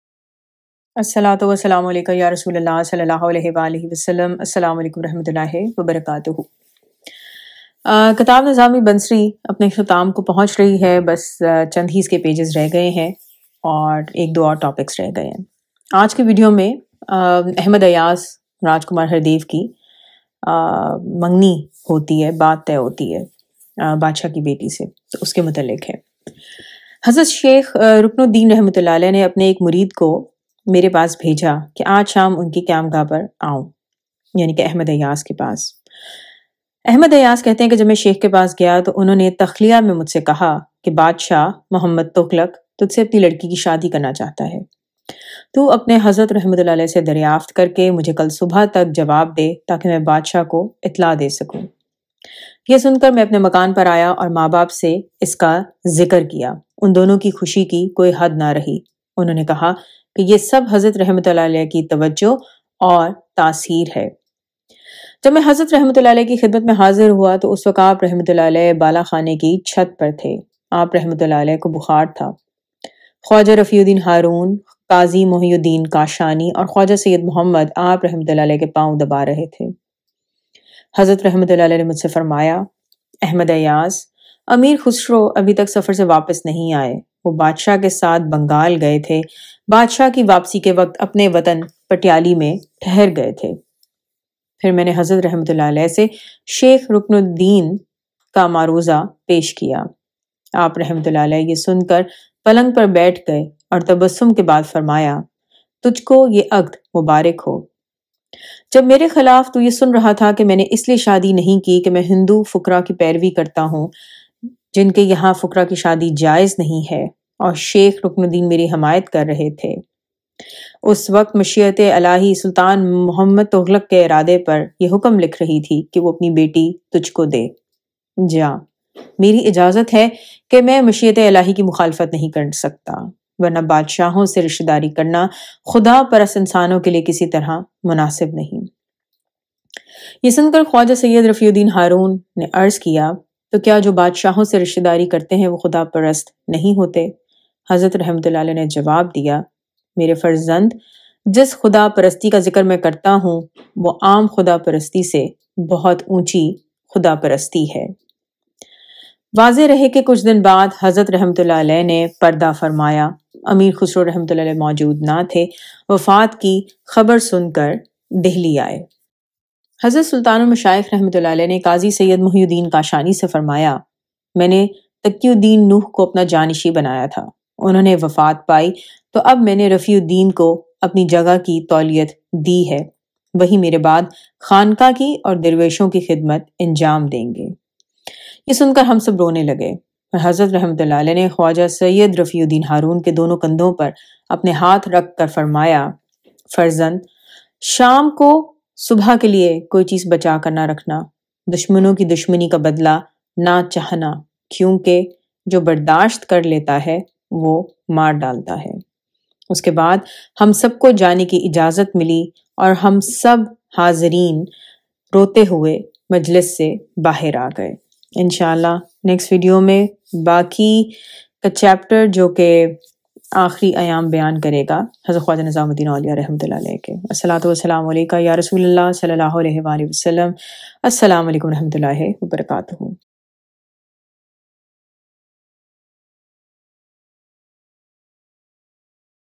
Video 44 | Nizami Bunsari | Life Story of Hazrat Nizamuddin RA | Ayaz ki shadi aur Kankah ka intezam
Click on the below link to access all videos of Nizami Bunsari | Life Story of Hazrat Nizamuddin Oliya Mehboob e Elahi RA